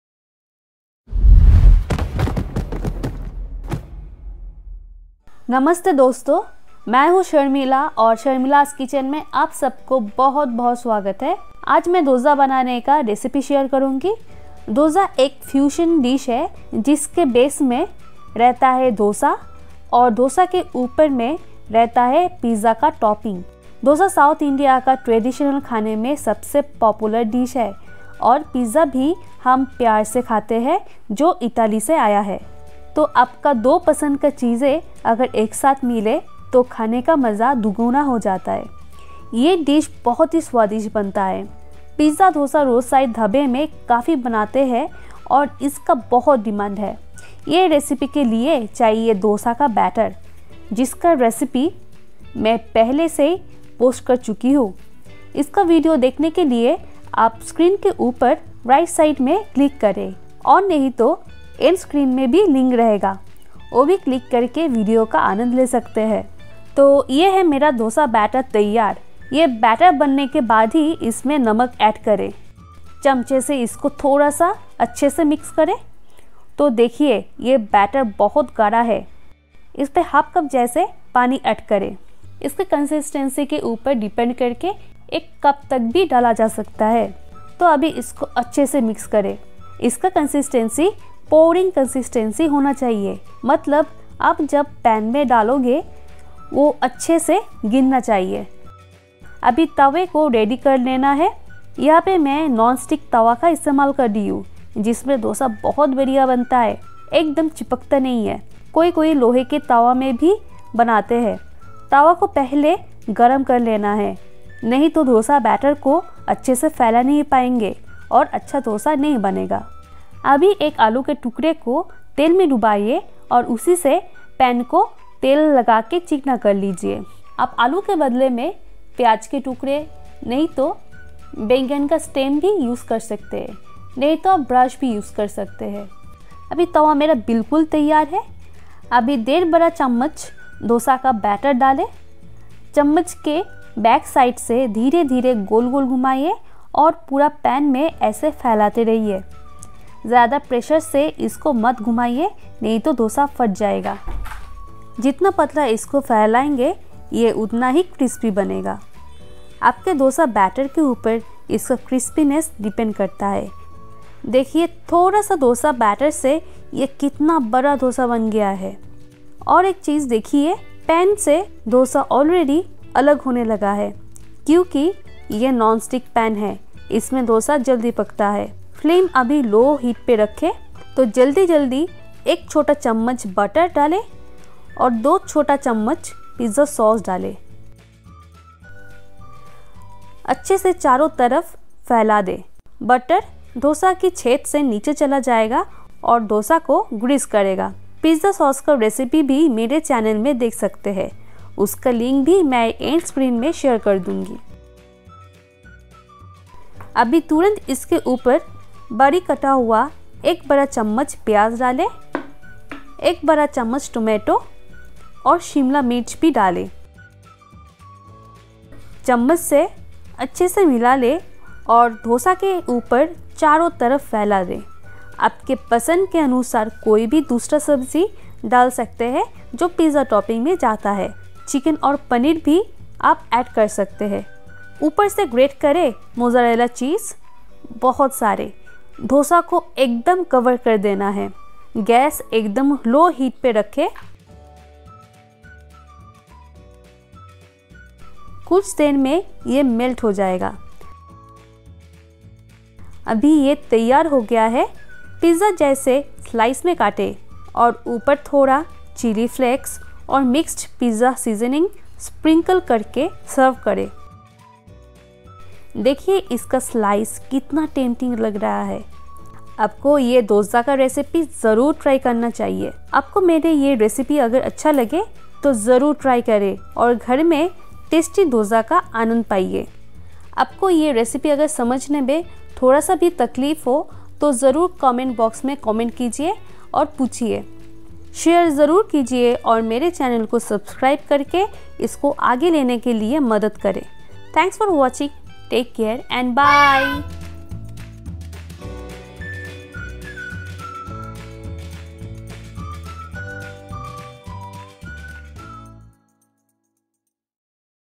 Dozza | Cheese Burst Pizza Dosa (With English Subtitle)- घर पे 5 मिनट में बनाइये पिज़्ज़ा डोसा
Today I made super hit recipe - dozza which is a combination of dosa and pizza topping.
Dosa is one of the popular dishes in South India and Pizza is also loved by all. If both of your favorite dishes combine together, then you would love it more.

1 tbsp Chopped onion, capsicum and tomato
1 cup Grated mozarella cheese to cover the dosa

More Dosa Recipe:
1. Plain Dosa, Masala Dosa and Onion dosa

Dosa Pan

THE ACCESSORIES I USED TO PRODUCE A VIDEO:
MY CAMERA
SOUND RECORDER
PHOTOGRAPHY LIGHT BOX